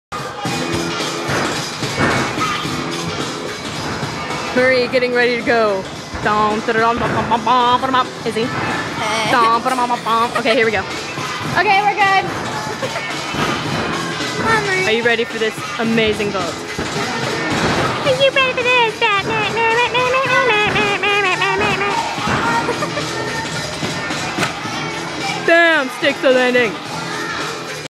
Yurchenko vault drill (with vocal effects by Miranda): mm gymnastics
My vault for lvl. 9 is going to be a yurchenko (haven't figured out tuck or pike yet...) Anyways, sorry about the background noise. It was my friend Miranda who was running on too little sleep. Ahhhh, gotta love early morning saturday practices...